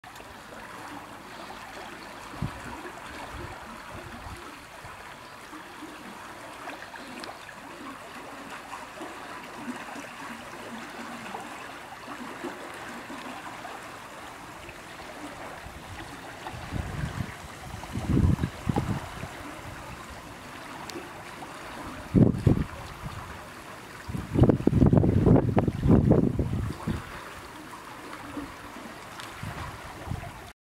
Balinok és kárászok
Balinok (Leucisus aspius) és ezüstkárászok (Carassius gibelius) Kis-Balatonra igyekvése március végén.